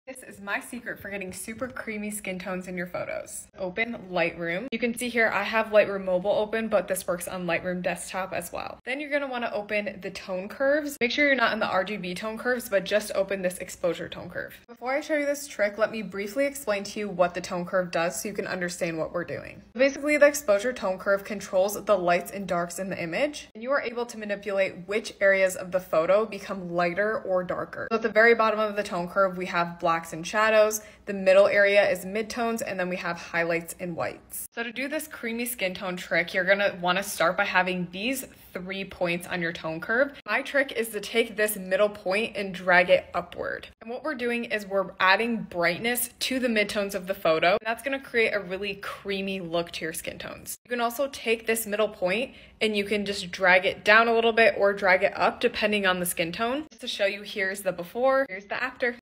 How To Get Creamy Skin Tones In Photos | Lightroom Tip
CC: Ever wondered how to get creamy skin tones? This is my secret for getting super creamy skin tones in your photos.

- So the first thing you're gonna wanna do is open Lightroom. You can see here I have Lightroom Mobile open, but this works on Lightroom Desktop as well. Then, you're gonna wanna open the tone curves. Make sure you're not in the RGB tone curves. But, just open this exposure curve tone.
- Before I show you this trick let me briefly explain to you what the tone curve does so you can understand what we're doing. So basically the exposure tone curve controls the lights and darks in the image and you are able to manipulate which areas of the photo become lighter and darker so at the very bottom of the tone curve we have blacks and shadows. The middle area is mid tones and then we have highlights and whites.
- So to do this creamy skin tone trick you're gonna wanna start by having these 3 points on your tone curve (as shown in video) and my trick is to take this middle point and drag it upward and what we're doing is we're adding brightness to the mid tones of the photo. That's gonna create a really creamy look to your skin tones. You can also take this middle point and you can just drag it down a little bit or drag it up depending on the skin tone. Remember that every skin tone is different so this point might need to be a little down or up, just depending.
- Here's the before and here's the after!